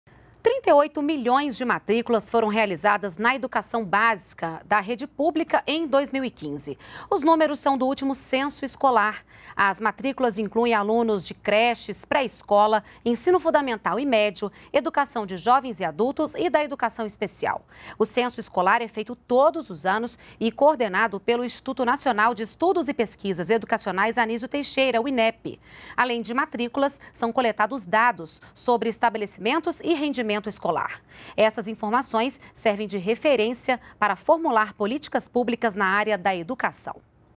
Em 2015, 38 milhões de matrículas foram realizadas na educação básica da rede pública
NBR NOTÍCIAS - 18.01.16: De acordo com os números do último Censo Escolar, mais de 38 milhões de matrículas foram realizadas na educação básica da rede pública em 2015. Fazem parte dessa pesquisa creches, pré-escolas, ensino fundamental e médio, educação de jovens e adultos, e da educação especial. O Censo, que é realizado todos os anos, é coordenado pelo Instituto Nacional de Estudos e Pesquisas Educacionais Anísio Teixeira, o Inep.